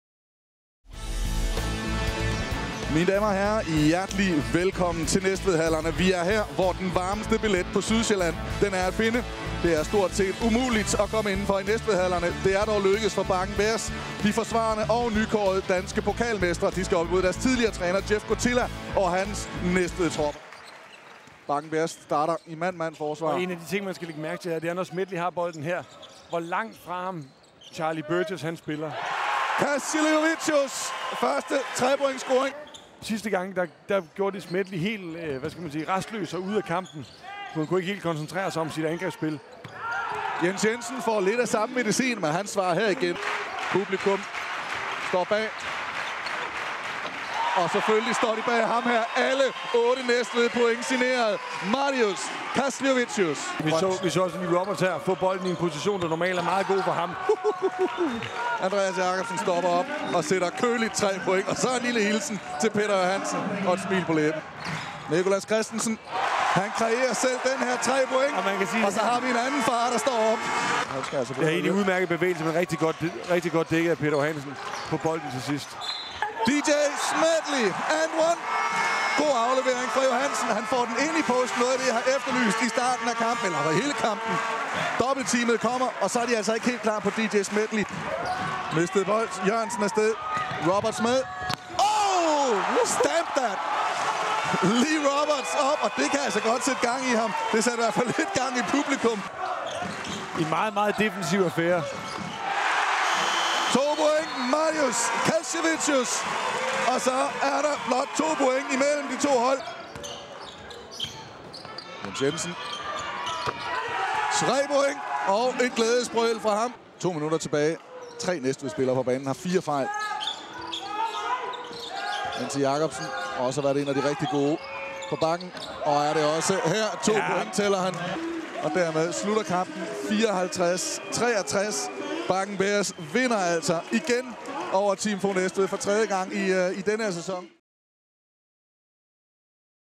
DK4 Highlights: Team FOG Næstved - Bakken Bears, 27. jan. 2011
Højdepunkter fra TV-kampen Team FOG Næstved - Bakken Bears
Husk du kan se basketball på dk4 hver torsdag kl. 19.00